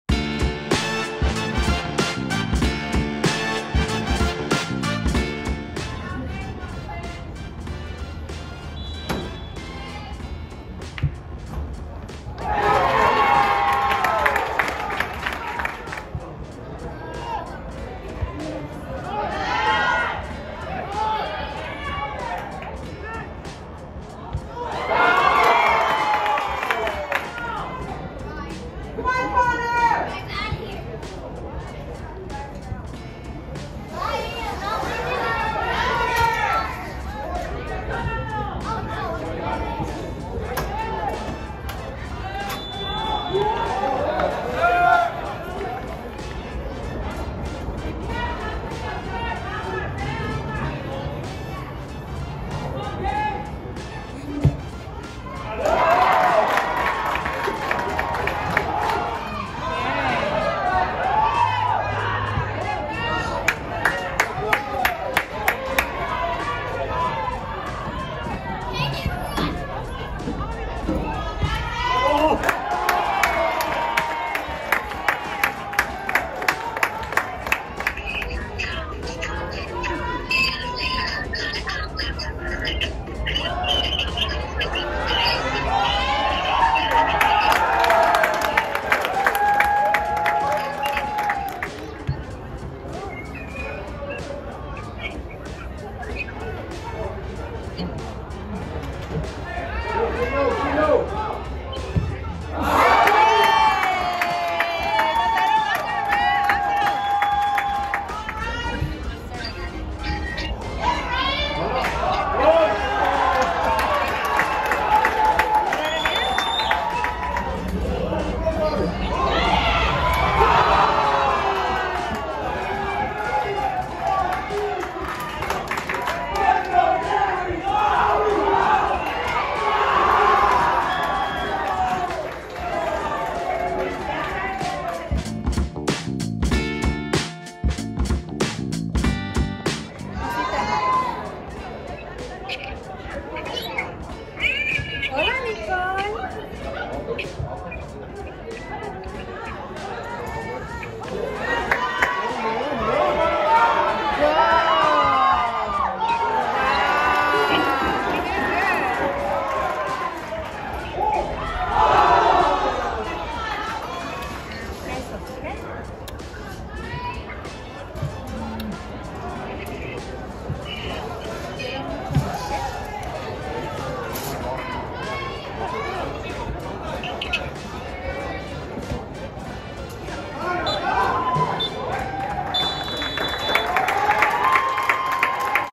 Highline high school vs Washington high school 8-0
Varsity High school soccer
5/10/2022